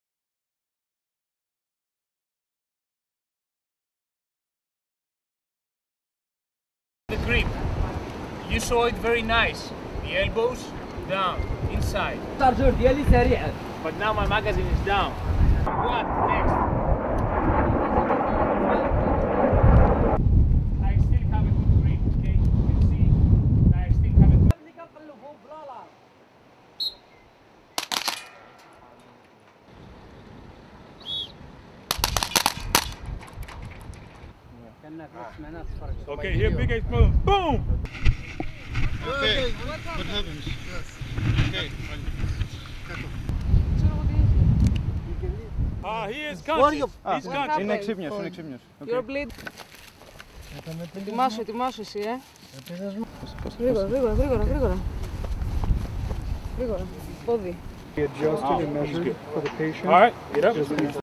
140515 Phoeonix Express 2014 B Roll
Naval personnel from multiple nations take part in the inport phase of Phoenix Express 2014.
(Video by MC3 Andrew Brame/Released)